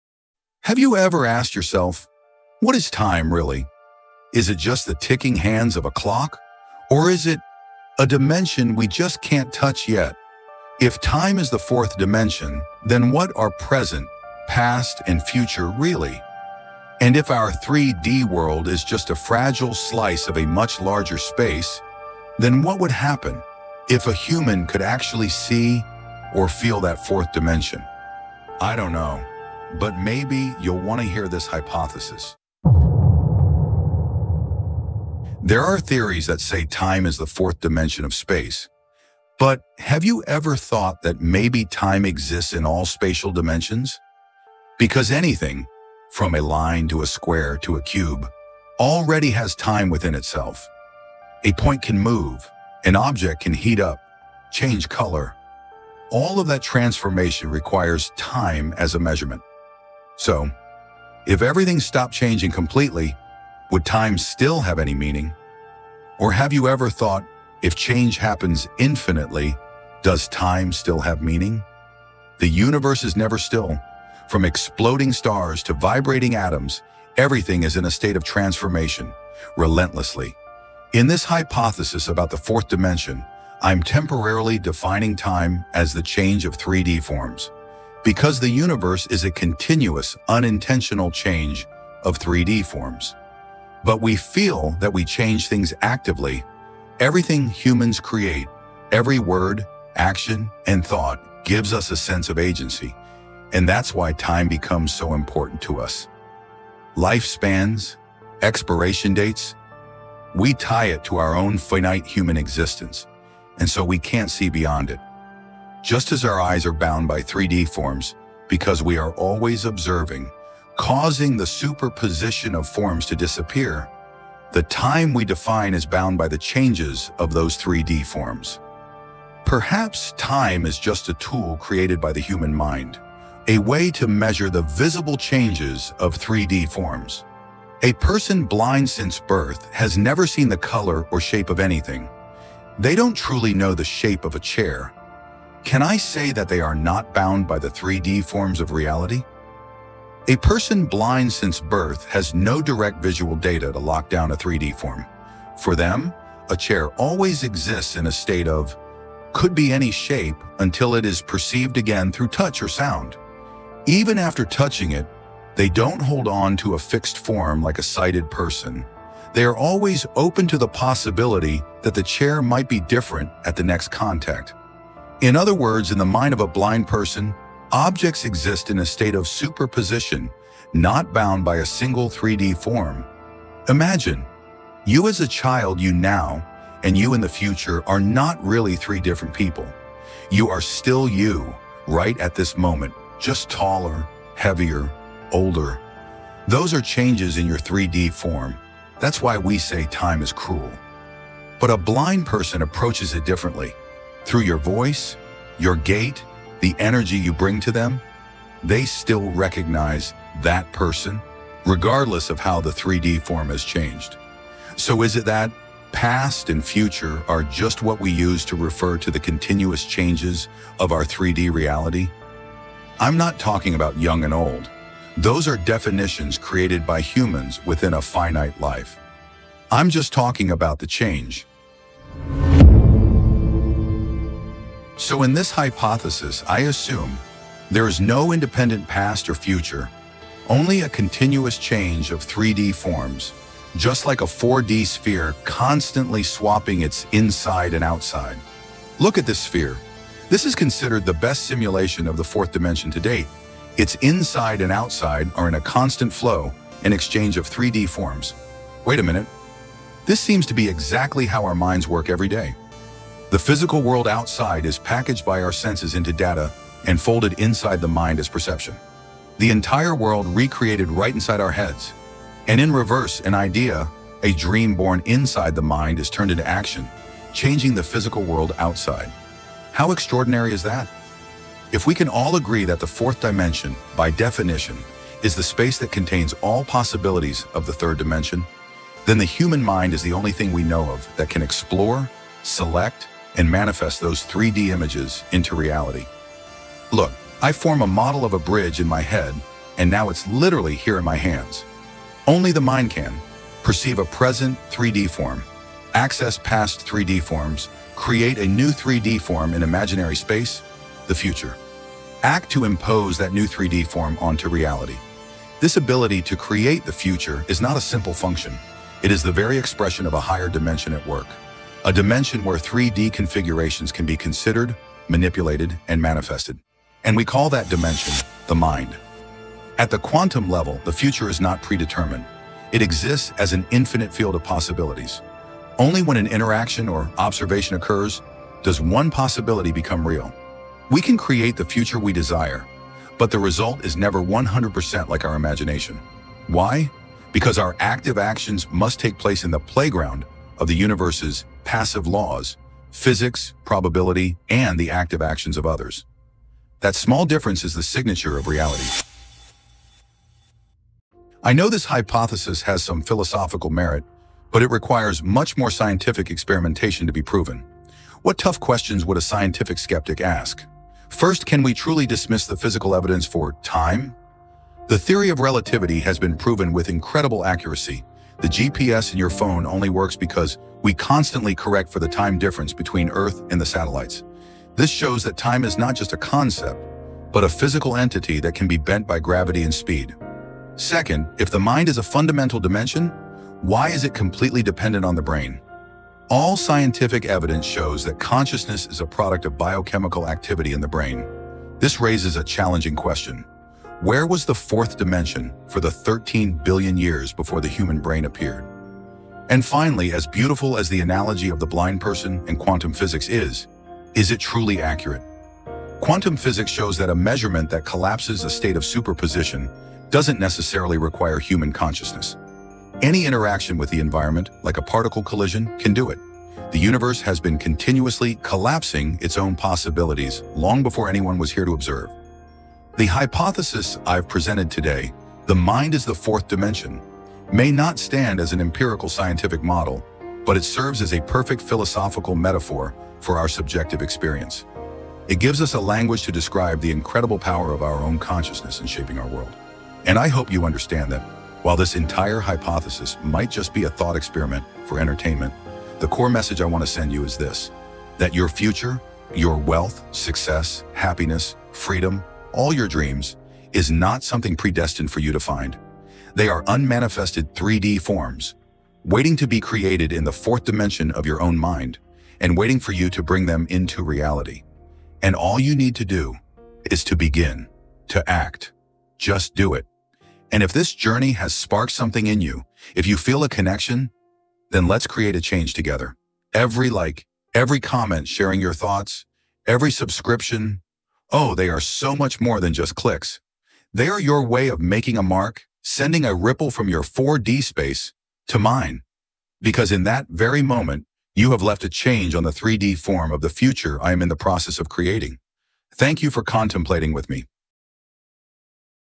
Hypothesis: The 4th Dimension is Your MIND (And You Use It Every Day)
What if our entire understanding of space and time is incomplete? The hypothesis in this video will change how you see reality: The fourth dimension is not Time. It's something closer, more familiar, and far more powerful—it is your MIND.

Discover how the mind functions like a 4D Tesseract, how the experience of the blind reveals the quantum nature of the universe, and most importantly, how you can use this very dimension to create the future you desire. This isn't just a theory; it's a guide to the hidden power within you.